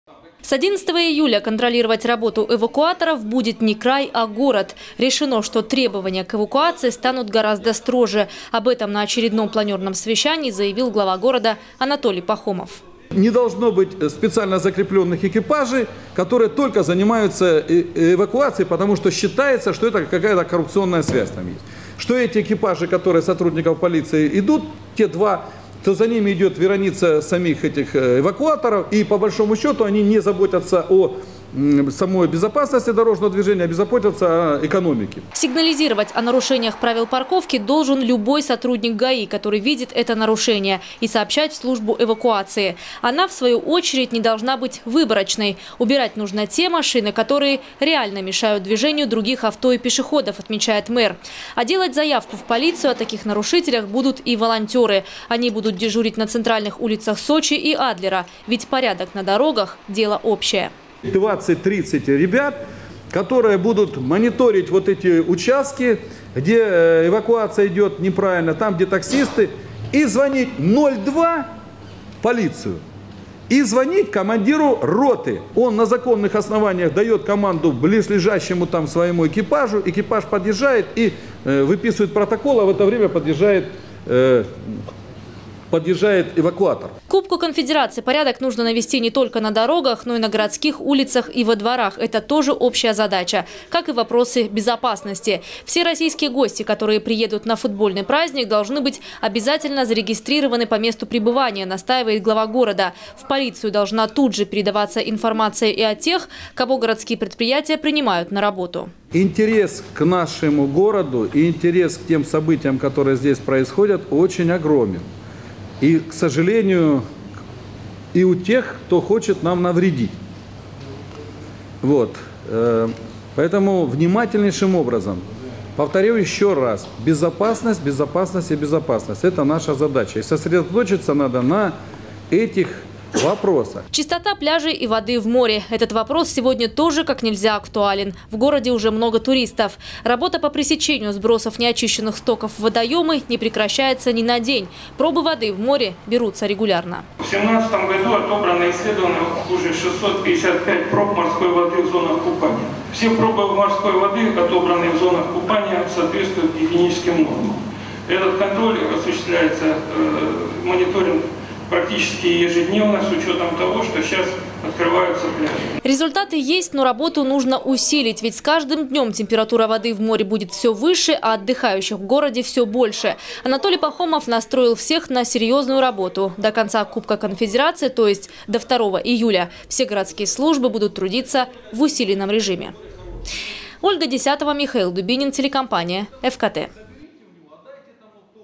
Волонтёры в Сочи помогут бороться с нарушителями правил парковки. Новости Эфкате
Такую идею сегодня на планёрном совещании озвучил глава города Анатолий Пахомов.

С 11 июля контролировать работу эвакуаторов будет не край, а город. Решено, что требования к эвакуации станут гораздо строже. Об этом на очередном планёрном совещании заявил глава города Анатолий Пахомов.

Сочи. Новости.
Телеканал Эфкате.